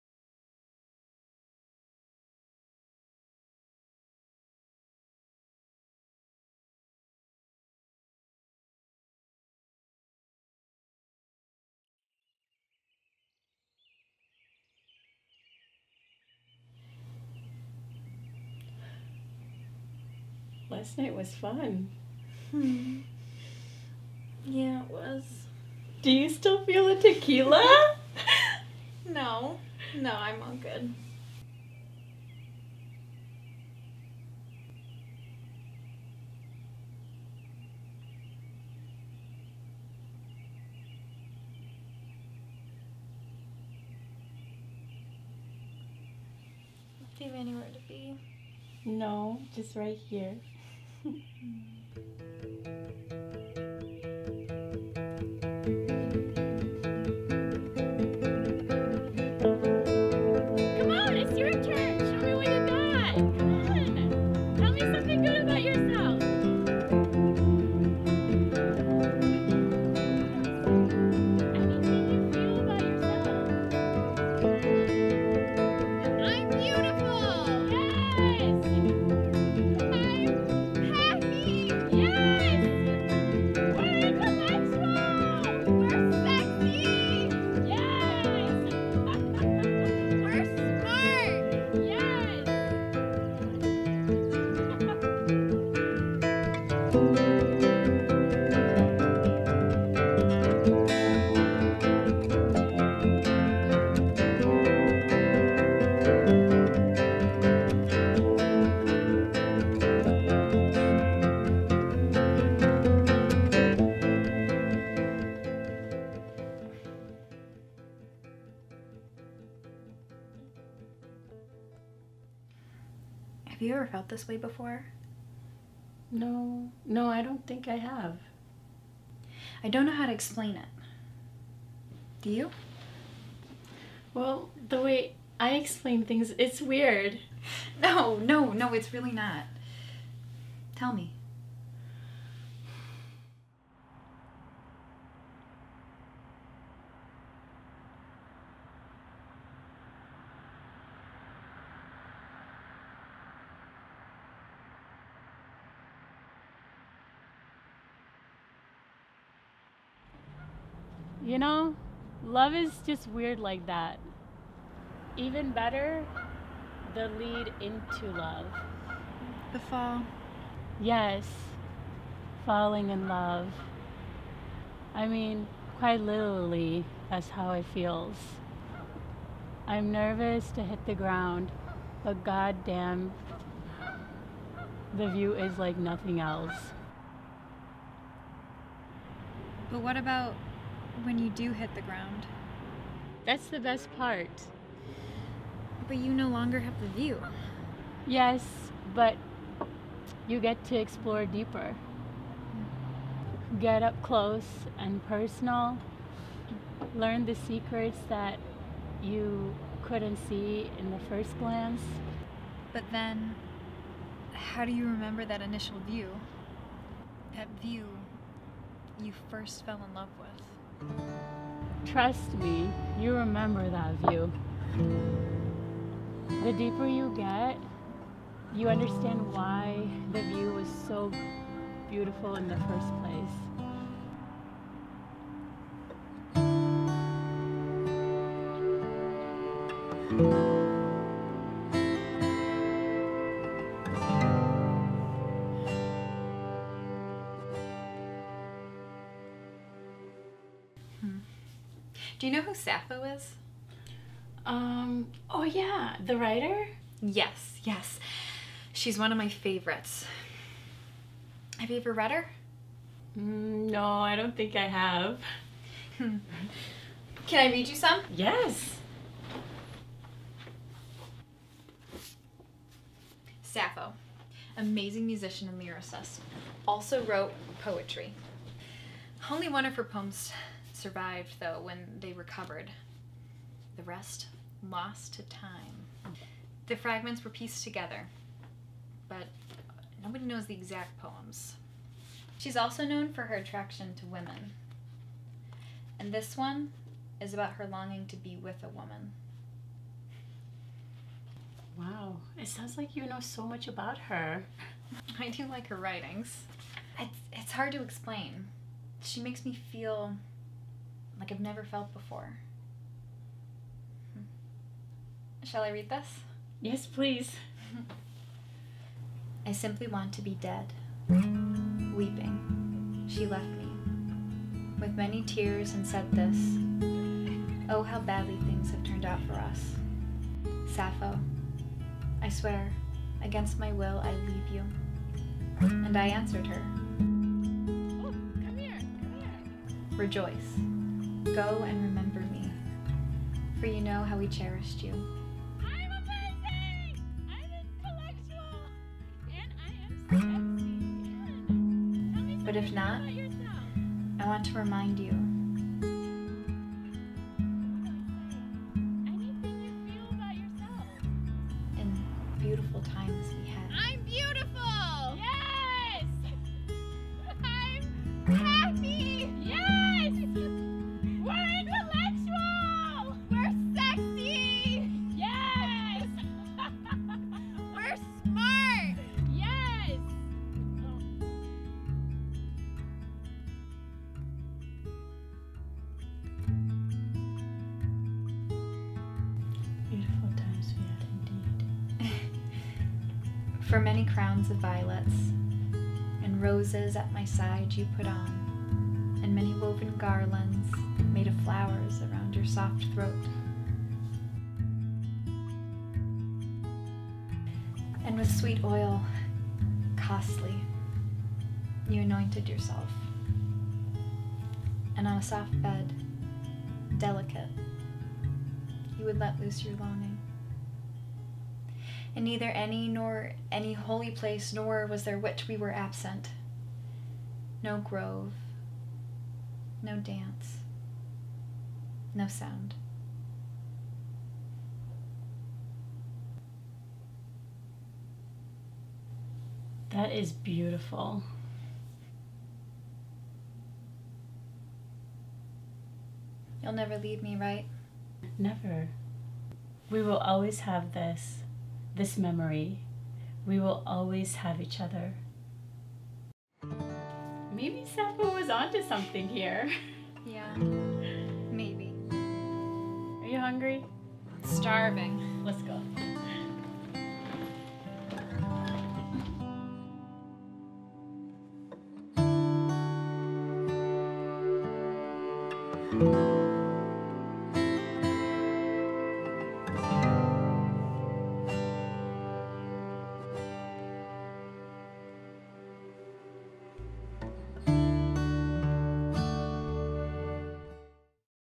A Glimpse Into Us
After a wild night, two women discuss their perceptions and experiences with love. Produced by Kelsey Peshek in Spring 2021.